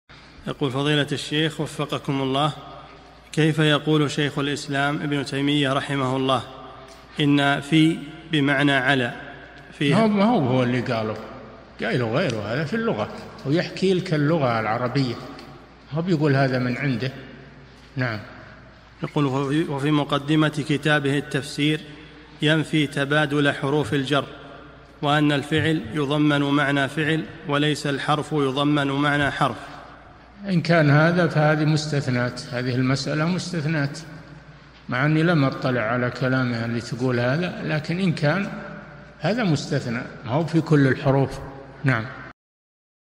[922 -1024] كيف الجمع بين قول ابن تيمية أن "في" بمعنى "على" وفي مقدمة التفسير، ينفي تبادل حروف الجر؟
-[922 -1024] السؤال: كيف يقول شيخ الإسلام ابن تيمية رحمه الله إن "في" بمعنى "على"، وفي مقدمة كتابه التفسير، ينفي تبادل حروف الجر، وأن الفعل يضمن معنى فعل، وليس الحرف يضمن معنى حرف؟
الجواب: ما هُوُ هُوَ الذي قاله، قائله غيره هذا في اللغة هو يحكي لك اللغة العربية ما يقول هذا من عنده السائل: يقول:  وفي مقدمة كتابه التفسير، ينفي تبادل حروف الجر، وأن الفعل يضمن معنى فعل، وليس الحرف يضمن معنى حرف؟ الشيخ: إن كان هذا فهذه مستثناة هذه المسألة مستثناة مع أني لم أطلع على كلامه الذي تقوله هذا، لكن إن كان فهذا مستثنى ما هو في كل الحروف.
للاشتراك في قناة الشيخ صالح الفوزان
سؤال وجواب من شرح الفتوى الحموية ( 1024 ) مقطع

للاستفسار والإقتراحات والدعم
رئيس الفريق
لمشروع كبار العلماء